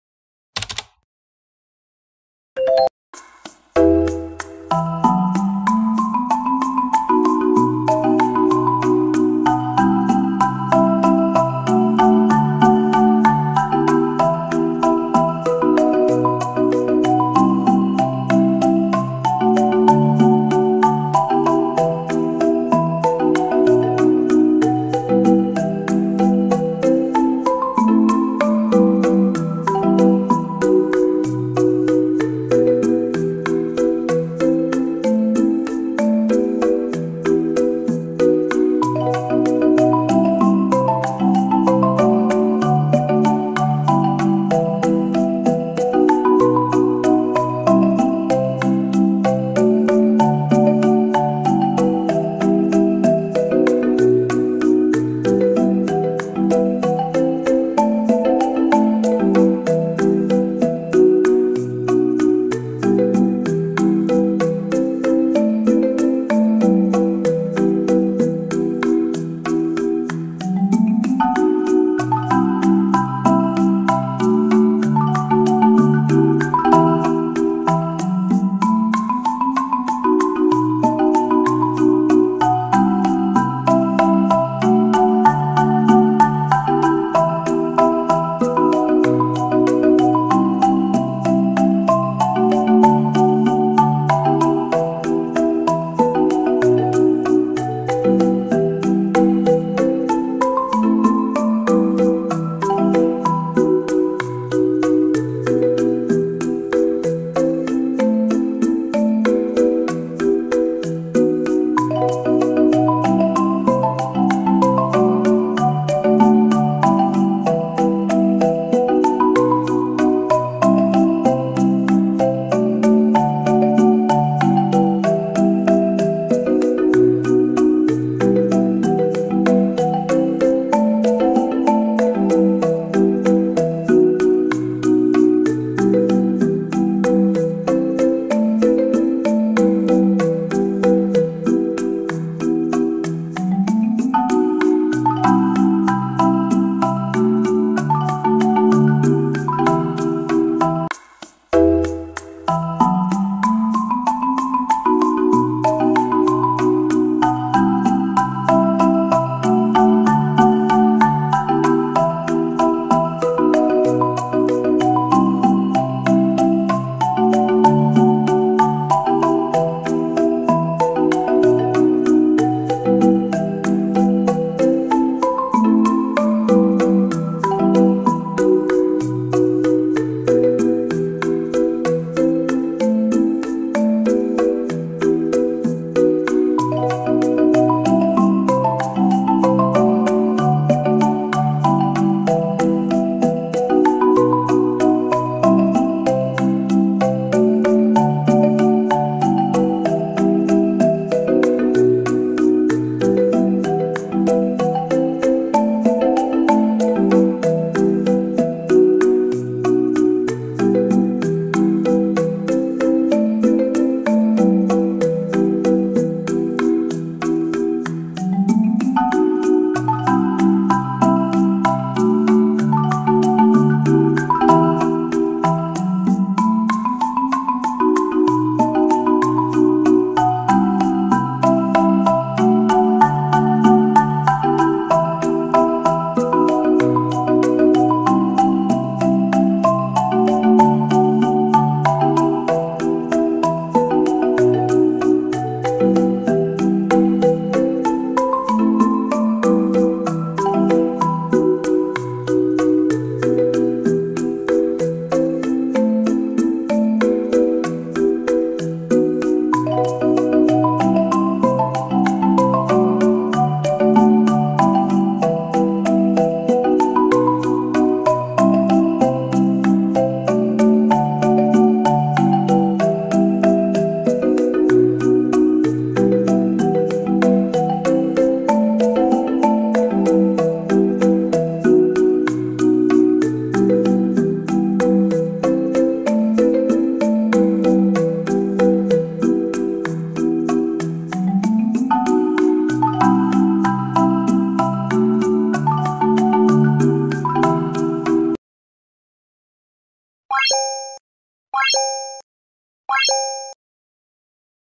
💖 Peacocks ladies sandals || June 2023 ✨Peacocks ladies shoes new in || Real Arifa
Come shopping with me at peacocks. In this video I'm sharing with you the latest women's shoes collection at peacocks.